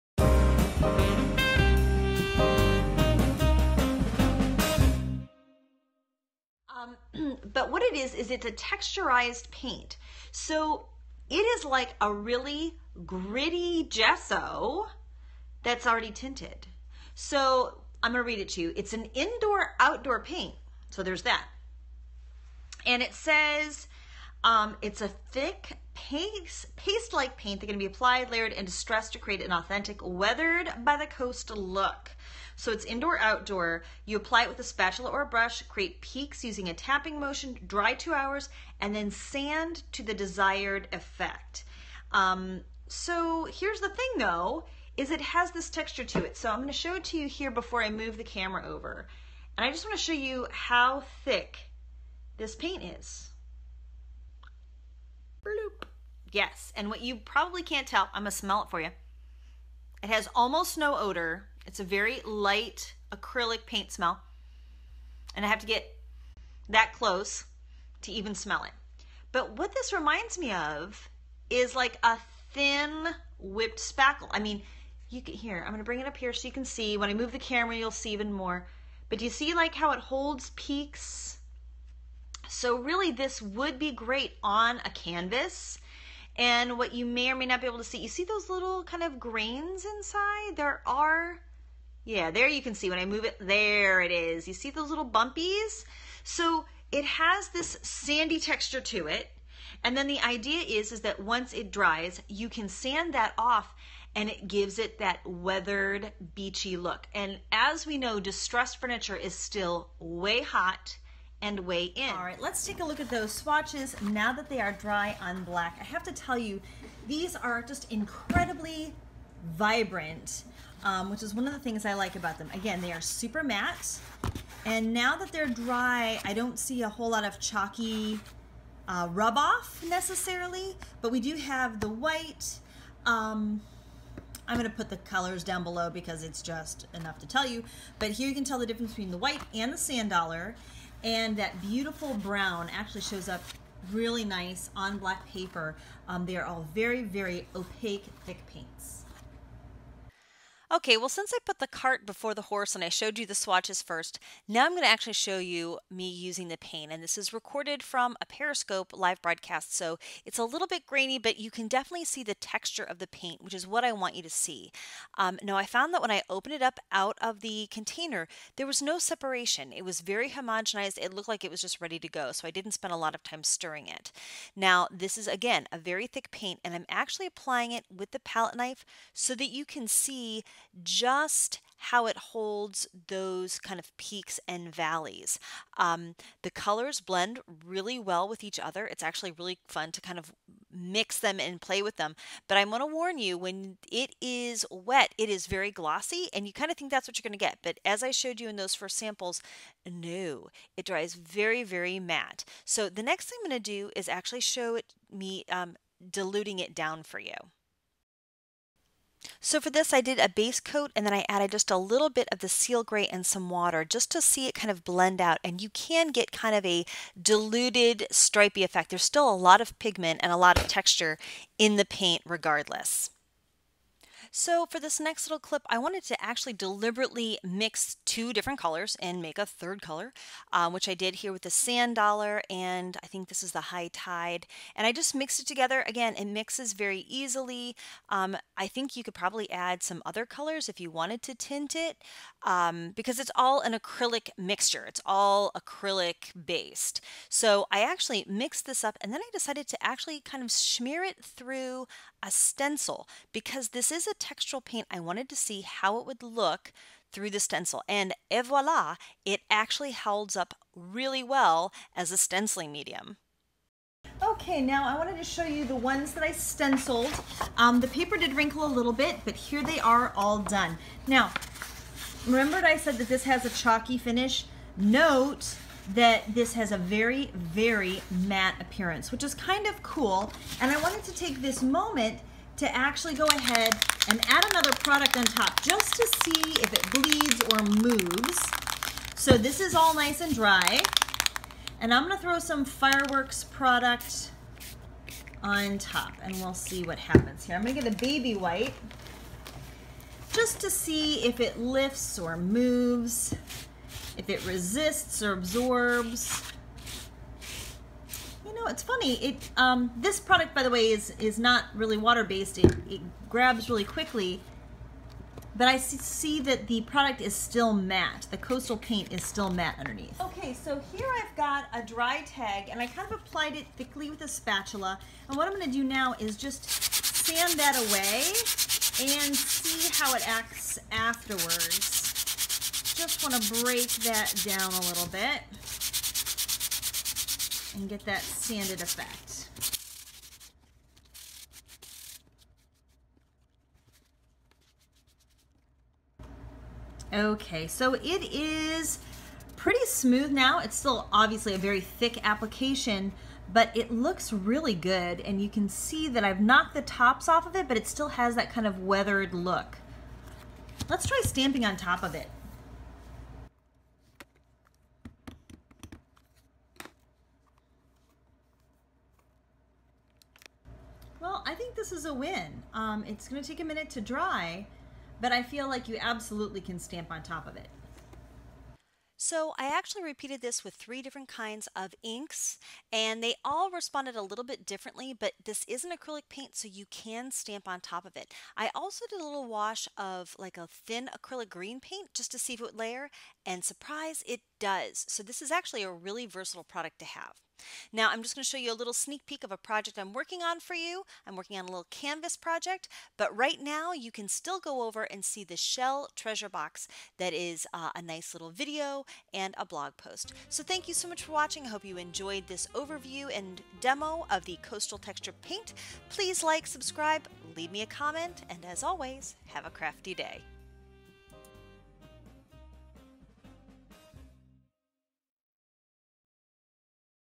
Coastal Texture Paint Review & Demo
Let's look at Coastal Texture Paint from Folk Art- it has the beach built in! This sanded, textured paint has body and is built to layer and sand for that "weathered coastal" look. But you can use it for paper, canvases, as well as home decor!